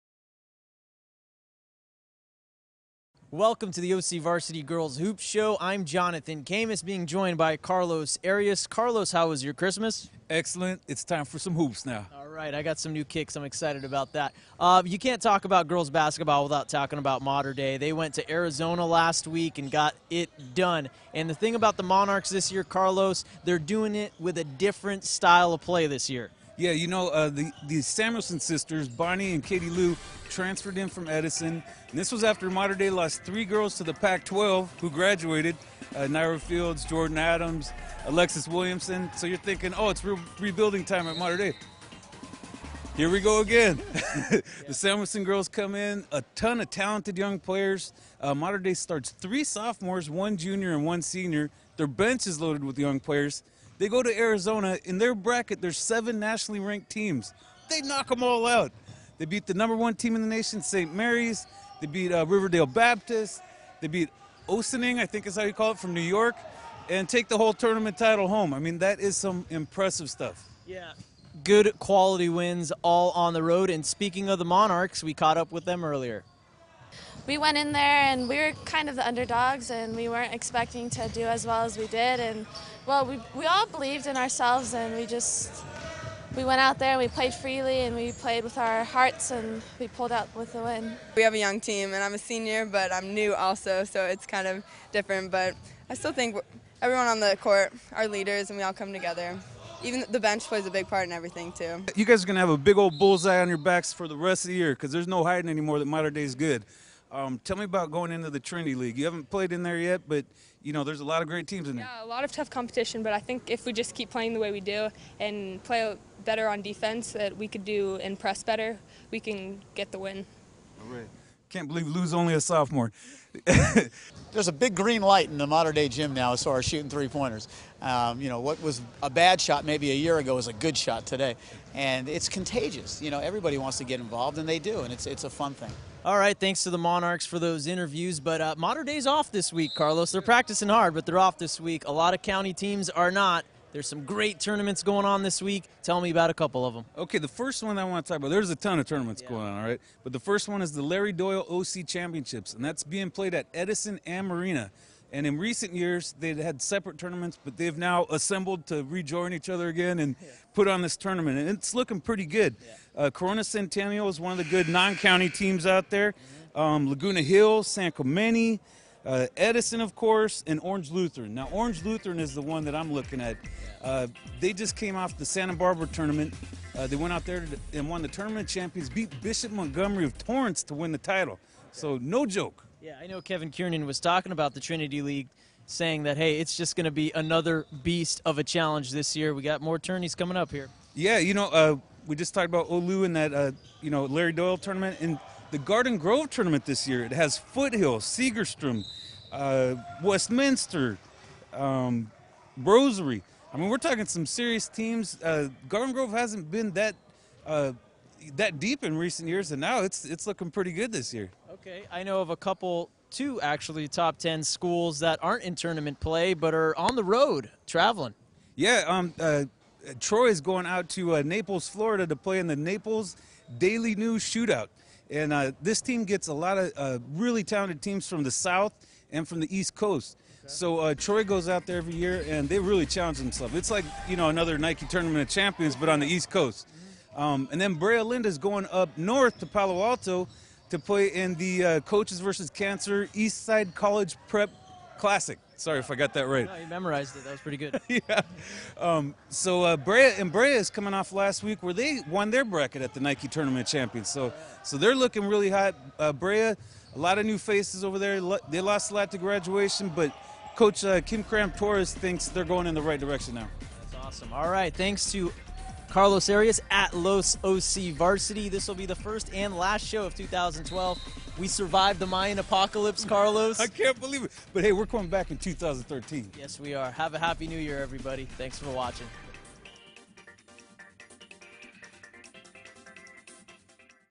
OCVarsity Girls Hoops: Holiday tournaments - 2012-12-27
Jonathan Khamis and Carlos Arias travel to Mater Dei to preview the upcoming holiday tournaments. Interviews with the Samuelson sisters, Kevin Kiernan, and Andee Velasco. Video by Jonathan Khamis, OCVarsity.com. Published on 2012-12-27